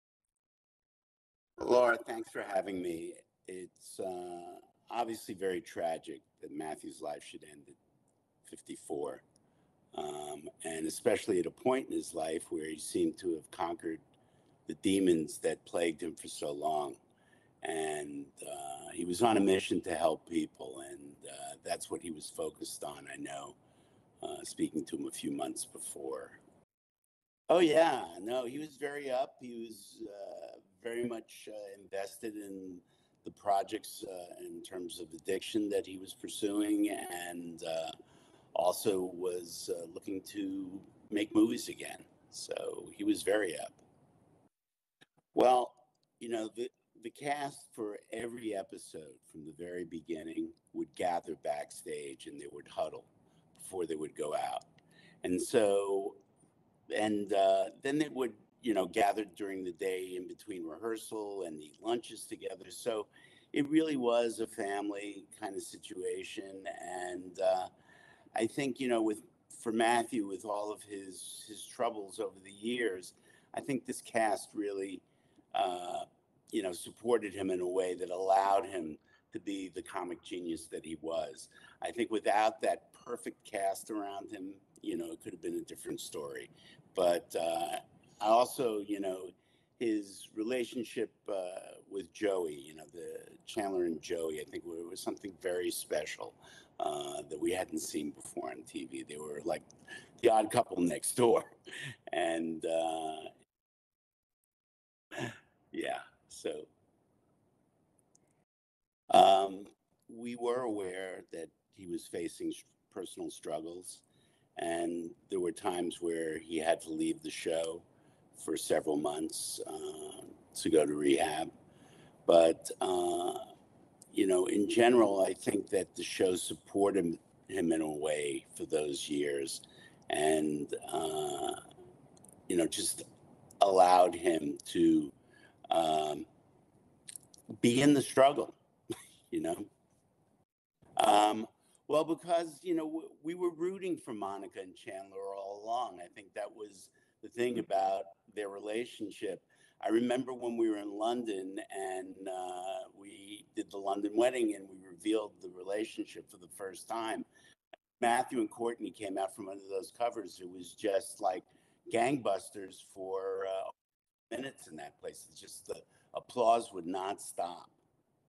‘Friends’ Executive Producer Kevin Bright Recalls Last Conversation With Matthew Perry
“Friends” executive producer Kevin Bright spoke with CNN about his last conversation with the late Matthew Perry, who starred as Chandler Bing in the television sitcom. “Obviously, very tragic that Matthew’s life should end at 54, especially at a point in his life when it seemed he had conquered the demons that had plagued him for so long,” he said.